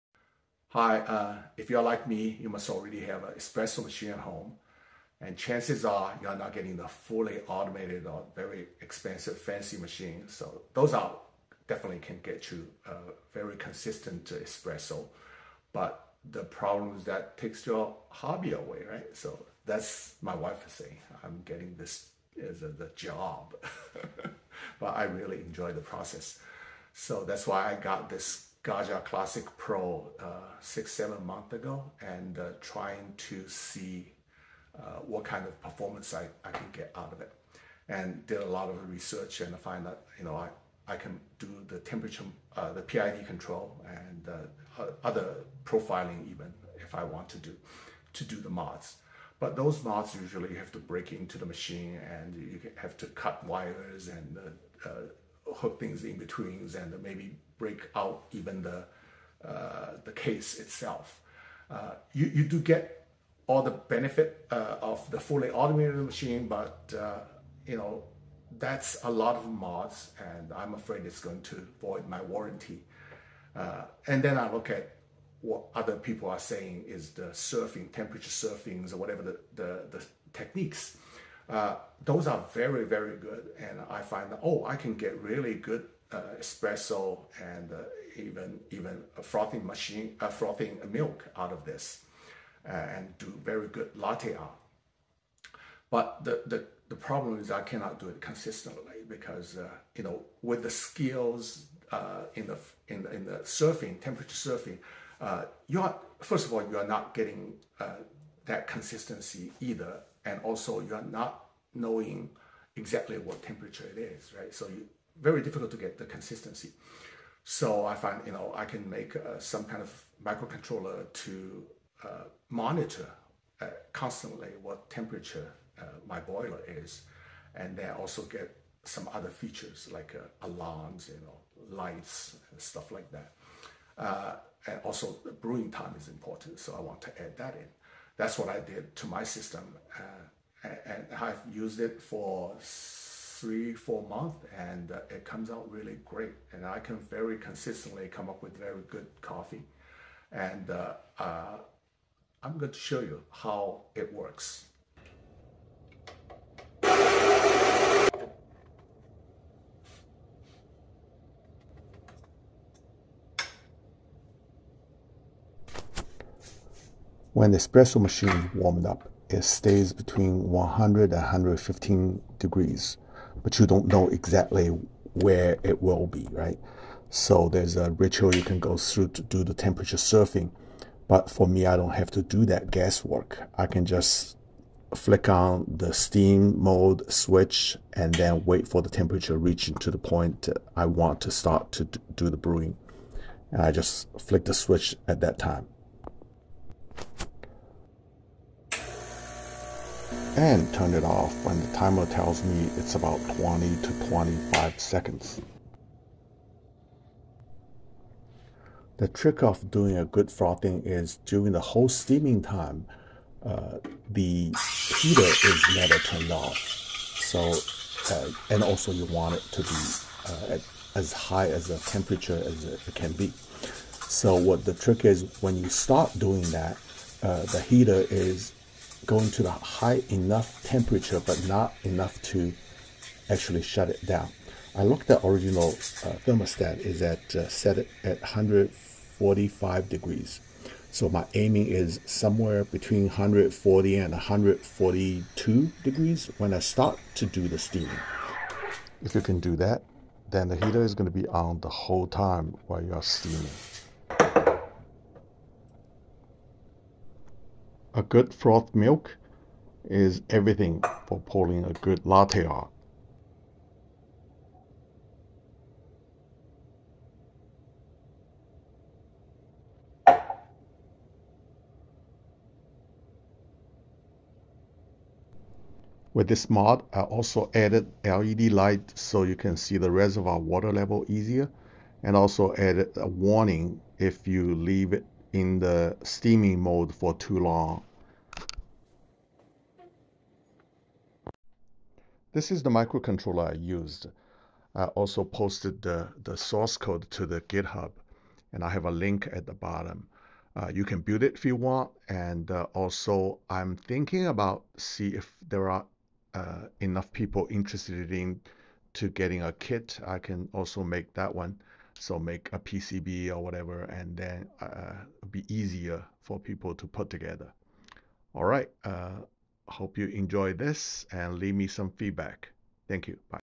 Non-intrusive Gaggia Classic Pro Espresso Machine Mod
A simple, non-intrusive mod to you espresso machine to allow you to monitor your boiler temperature and brewing time along with alarm when you left your machine in steaming mode for too long.
esp8266 is the micro-controller used and the its source code can be found in GitHub